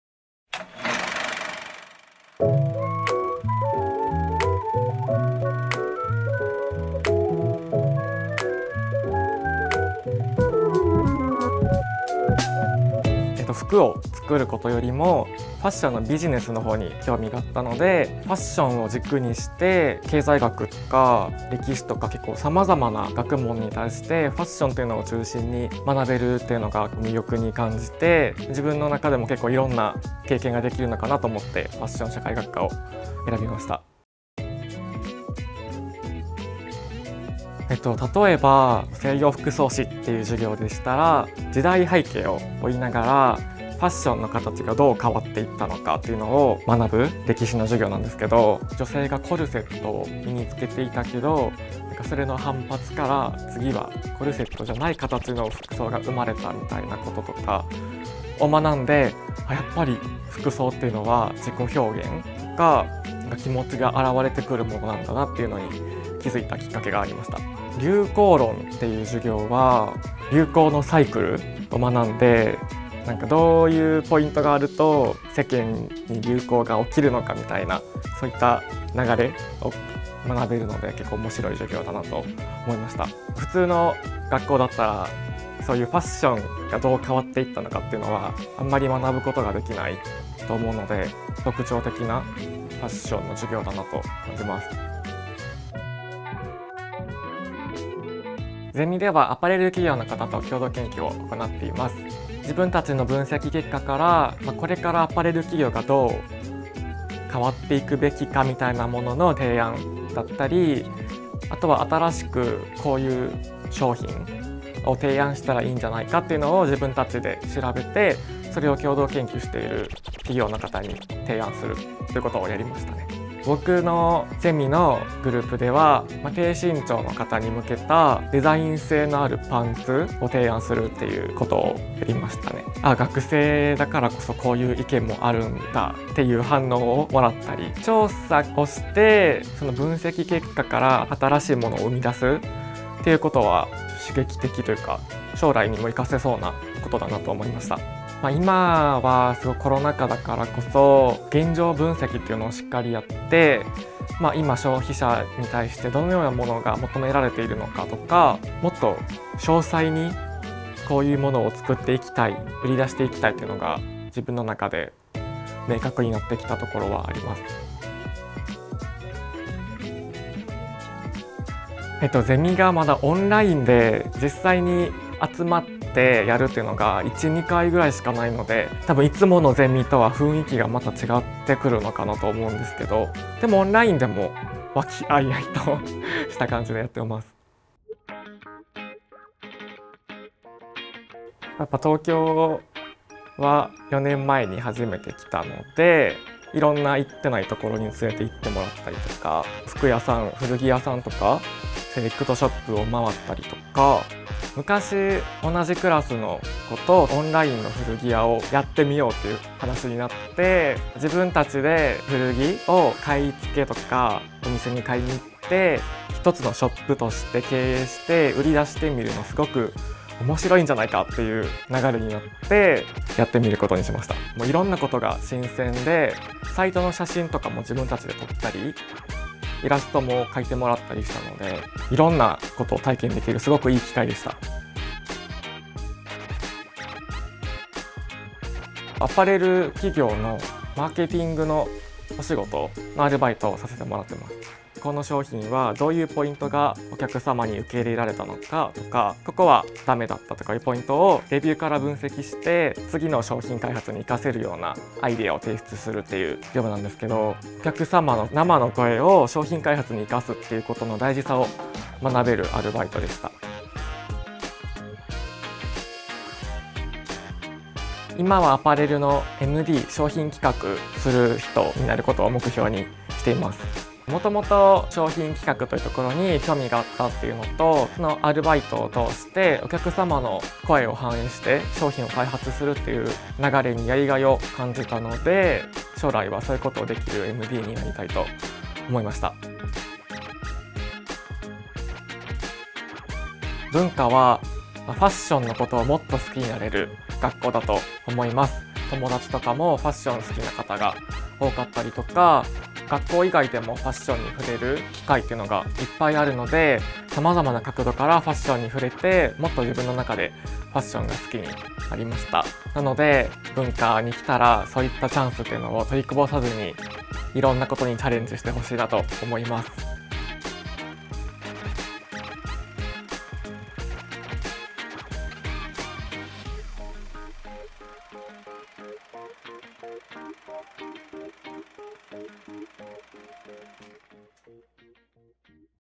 文化学園大学｜在学生インタビュー ファッション社会学科
【在学生インタビュー】
ファッション社会学科4年生（インタビュー時）に大学生活を語ってもらいました。

「経済学や歴史などさまざまな学問に対して、ファッションを中心に学べることに魅力に感じて」ファッション社会学科へ。
授業以外にも、友達と古着のオンラインストアを運営したり、マーケティングのアルバイトをしたり…さまざまな場所で学びを生かしているそう。

- こんなことを聞きました！ -

00:10　ファッション社会学科を選んだ理由
00:35　印象に残っている授業は？
01:35　今はどんなことを学んでいる？
02:49　ゼミの雰囲気はどう？
03:08　友達とのエピソード
03:56　アルバイトは何をしている？
04:25　将来の夢
04:50　受験生へのメッセージ